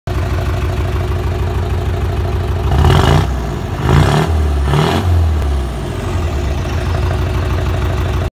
Land Rover Defender 300 TDI exhaust
The old 2.5 TD has been replaced with a freshly rebuilt 300 TDI.  The straight through exhaust sure let’s us hear that turbo.